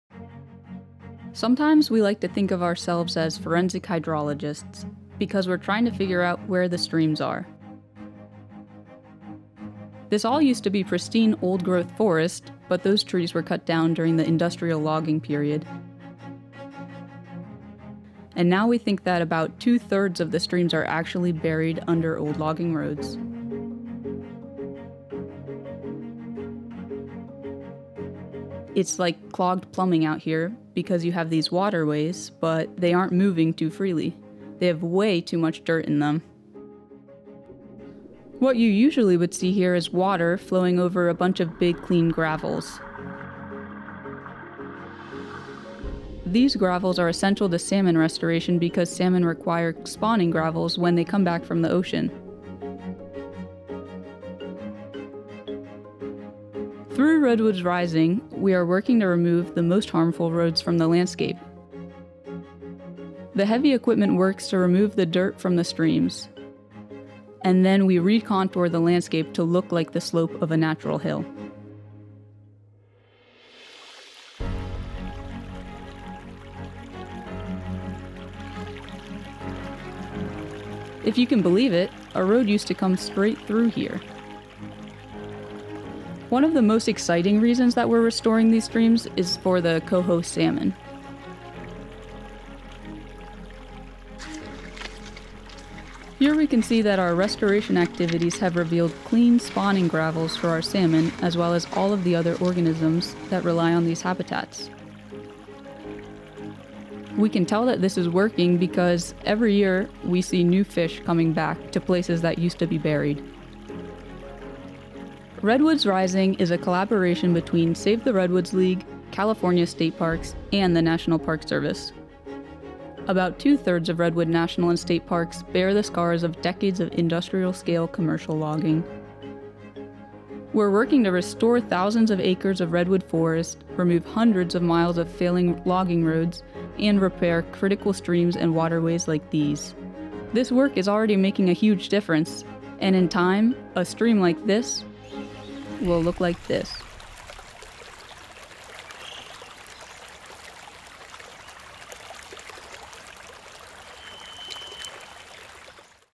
Daylighting Hidden Streams with Redwoods Rising | Save the Redwoods League
🌲 Restoring Redwood Streams🐟

What happens when decades of industrial logging bury vital waterways beneath old roads? Follow the work of the Redwoods Rising initiative—a collaboration between Save the Redwoods League, California State Parks, and the National Park Service—as they uncover and restore hidden streams in California’s redwood forests.

🔍 Acting like "forensic hydrologists," these restoration experts are:

- Revealing buried streams once clogged by logging roads
- Removing sediment to restore natural water flow
- Recontouring landscapes to mimic natural hillsides
- Reintroducing clean spawning gravels essential for coho salmon

💧 Why it matters: These efforts are critical for salmon populations and the countless organisms that depend on healthy stream ecosystems. And the results are already visible—fish are returning to places that were once buried.

🌿 Learn how thousands of acres of redwood forest are being healed and how nature is bouncing back, one stream at a time.

📽️ Watch now to see how Redwoods Rising is reviving the redwoods.

📣 Share: Spread the word on social media to inspire others.